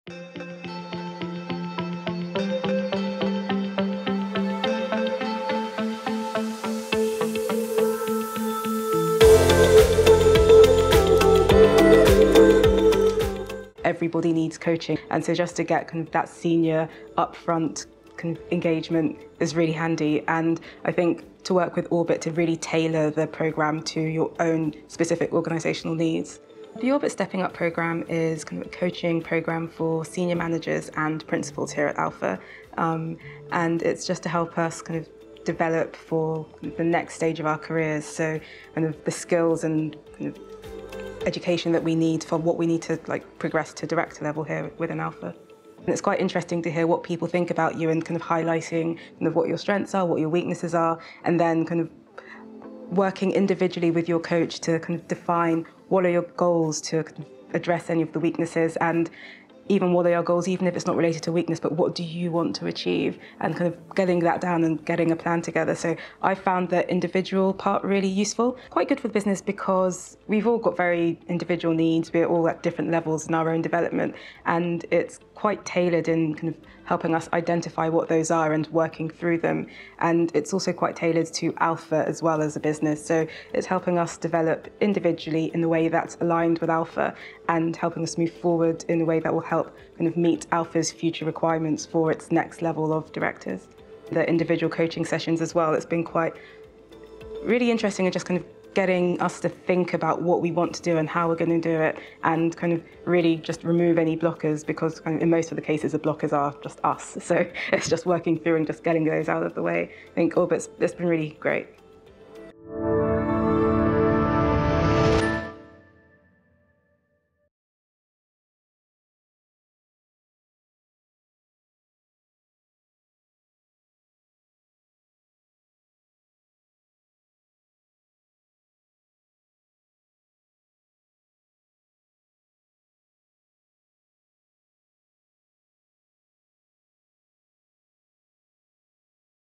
COACHING: Tailored to the individual
Zenabu Labri from Alpha FMC talks about what Orbit's tailored coaching is really like.

We help you achieve your business objectives by putting the customer at the centre of what you do.